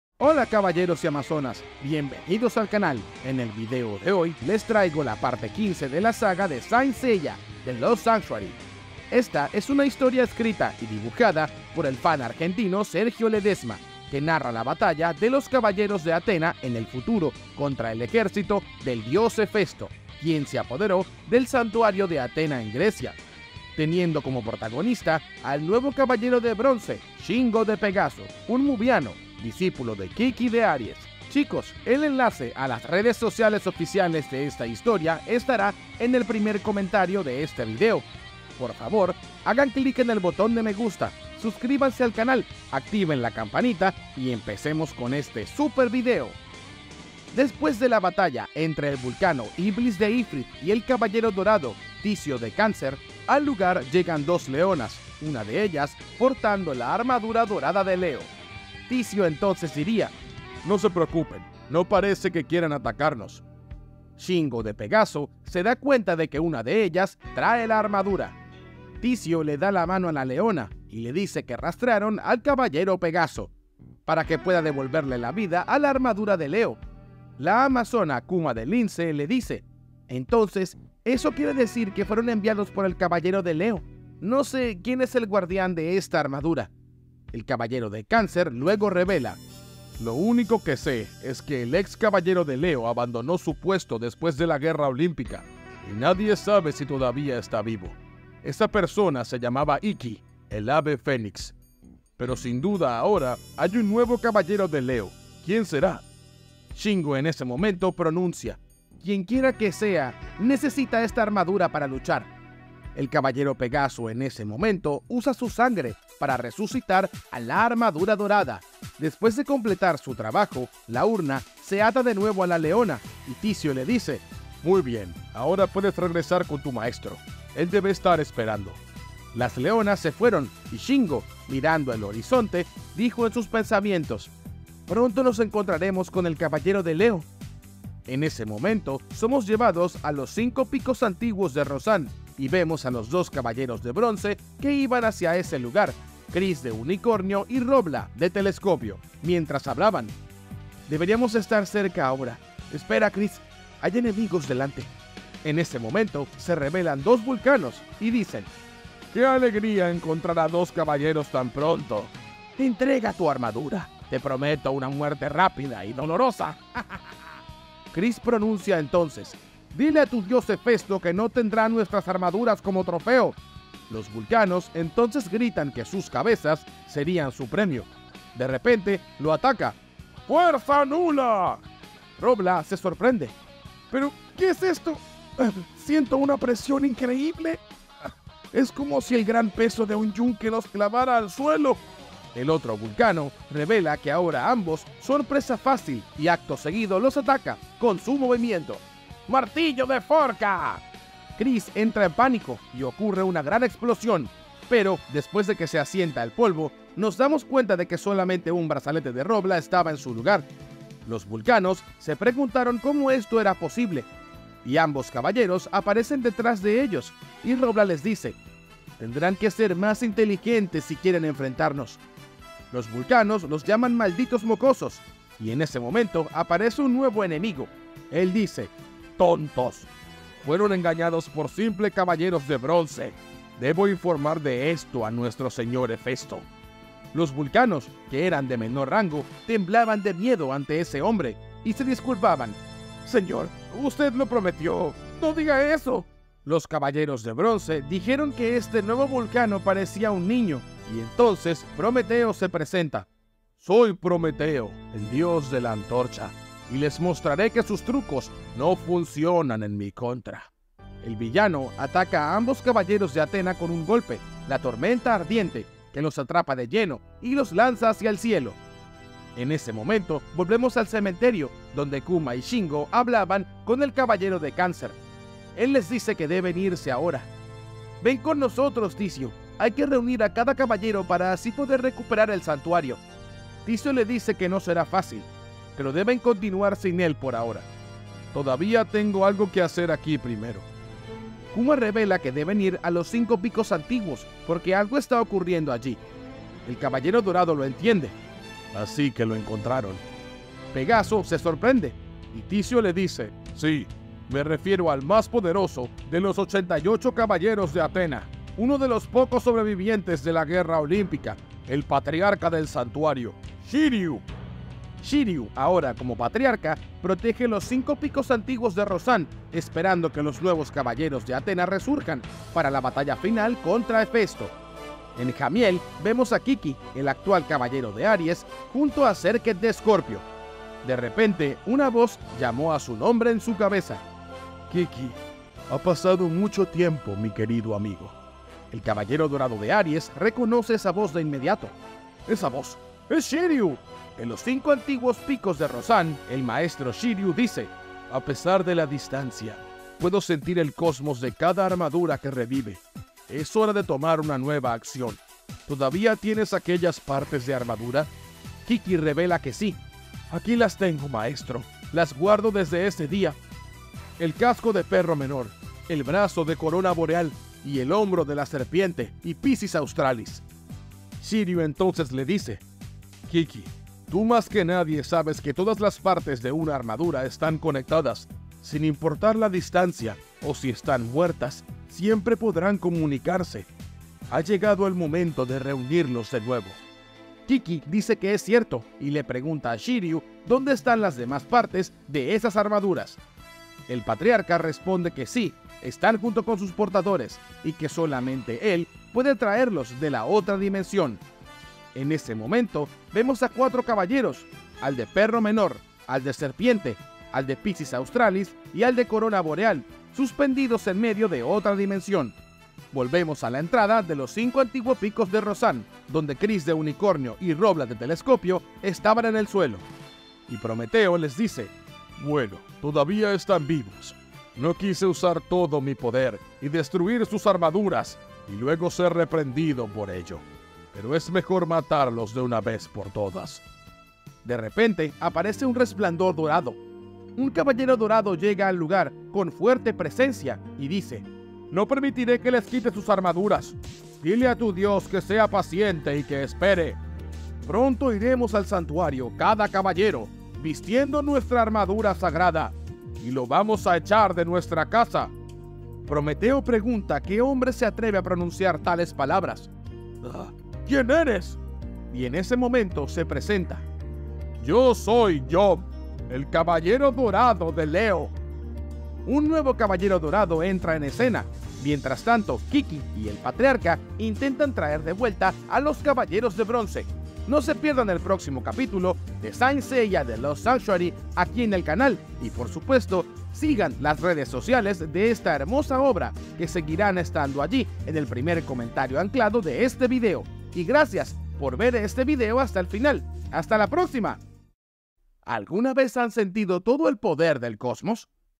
¡REVELÓ! ¡SHIRYU es el NUEVO PATRIARCA DEL SANTUARIO! Saint Seiya Lost Sanctuary / Parte 15
En este video les presento el Manga Saint Seiya Lost Sanctuary.
Esta historia está escrita y dibujada por Sergio Ledesma.

Video Categoría: Curiosidades de Saint Seiya.

Saint Seiya es una obra original de Masami Kurumada.